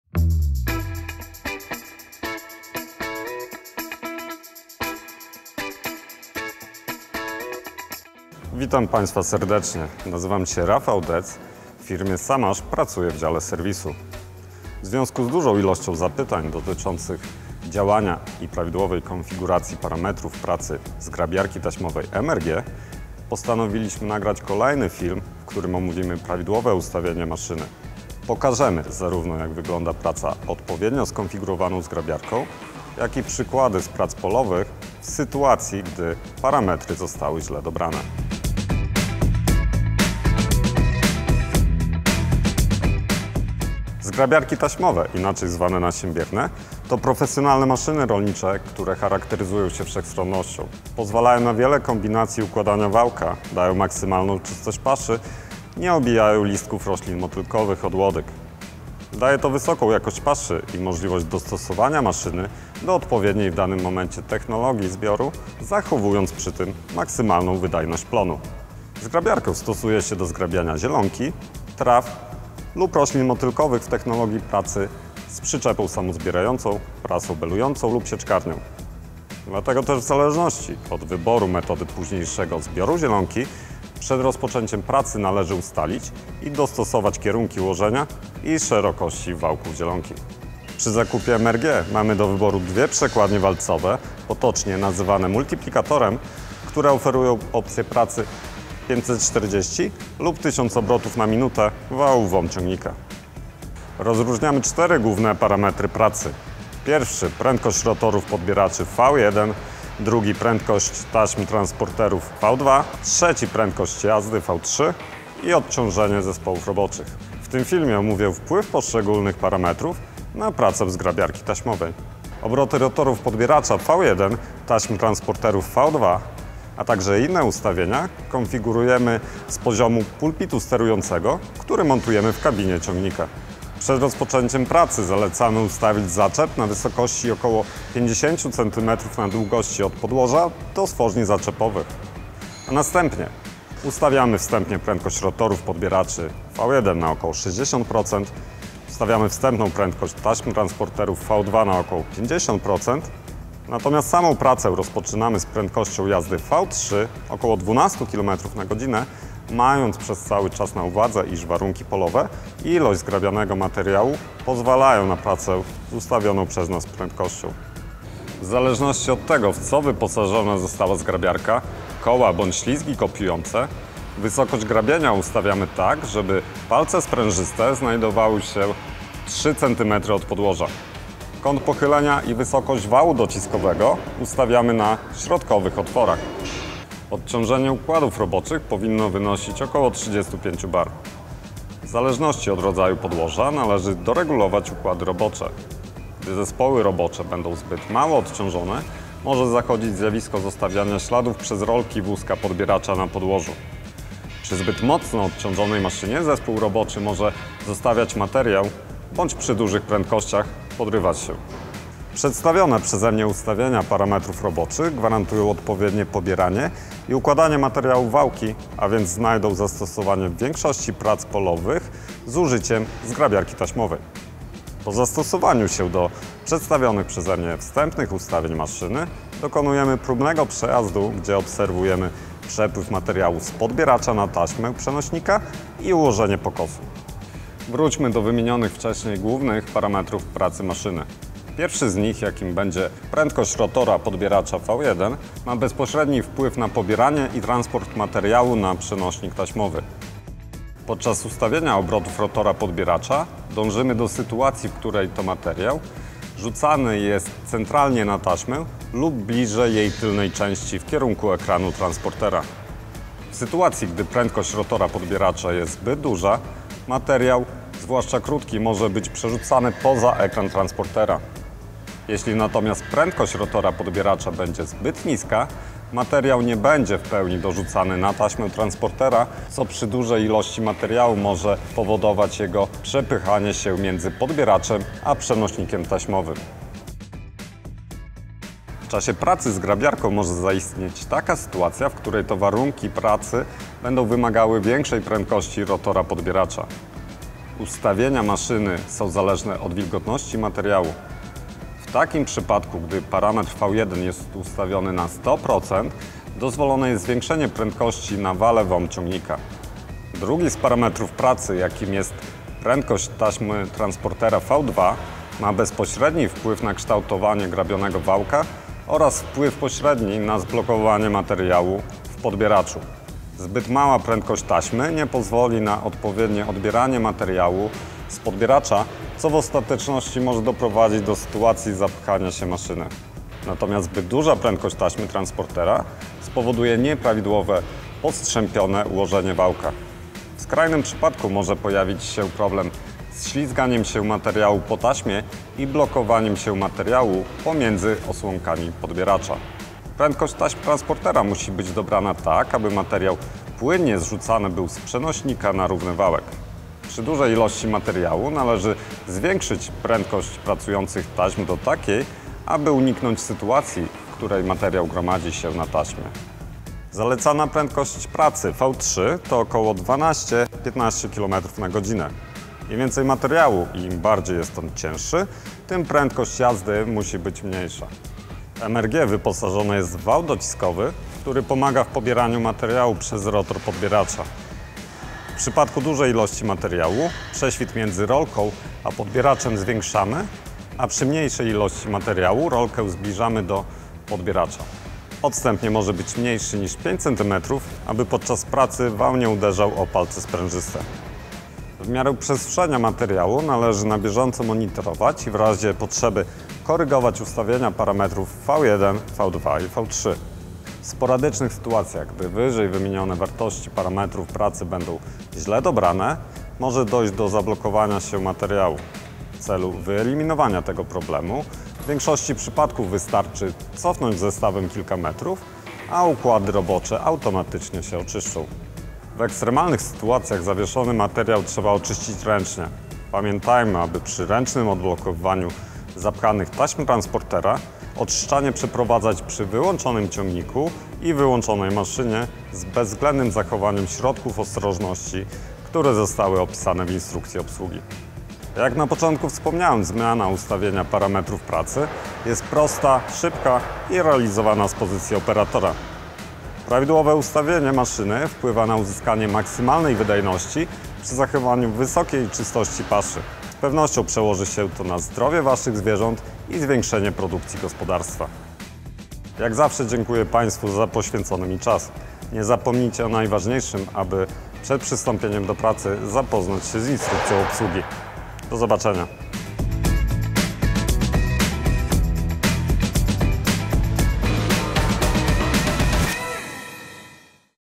SaMASZ merger 2-900 set up / ENG subtitles
MRG 2-900 belt rake is a professional machine designed for service providers and large-scale farms.

Advantages of MRG 2-900 belt rake:

🌿 the proprietary cam pick-up design guarantees the pick-up of any kind of material regardless of its length
🌿 belt conveyor allows transport of mowed material in the direction of the operator's choice
🌿 ability to collect forage from a large acreage in a short period of time
🌿 cleanest and most efficient harvesting of material